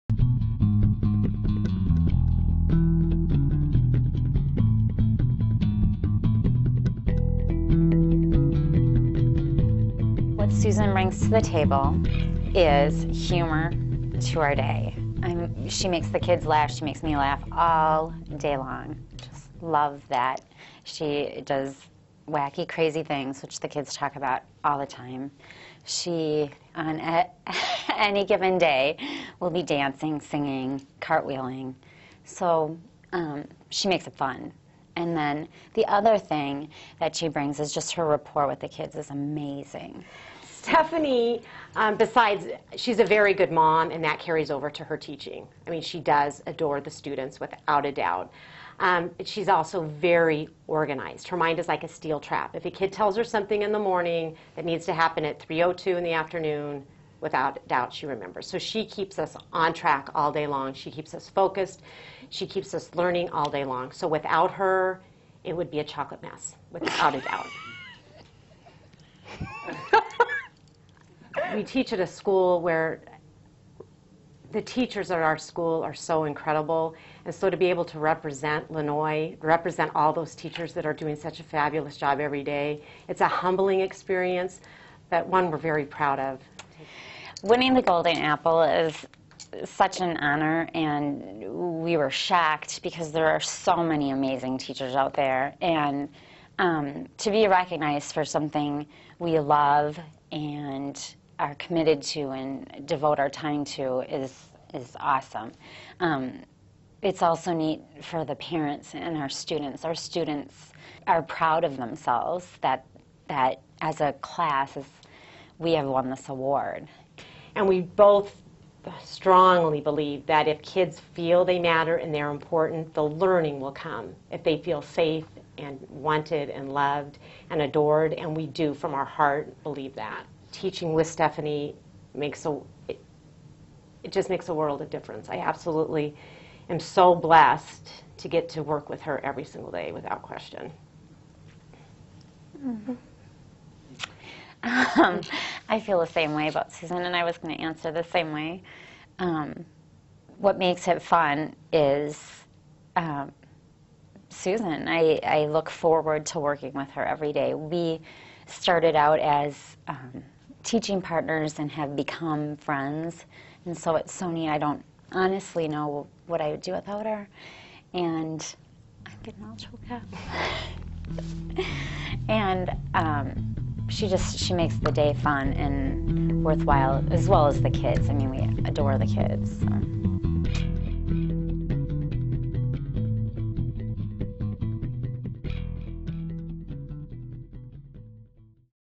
2012 Golden Apple Award: Lannoye Elementary teachers Stephanie Heezen & Susan McMahon
Lannoye Elementary School teachers Stephanie Heezen and Susan McMahon win 2012 Golden Apple Award.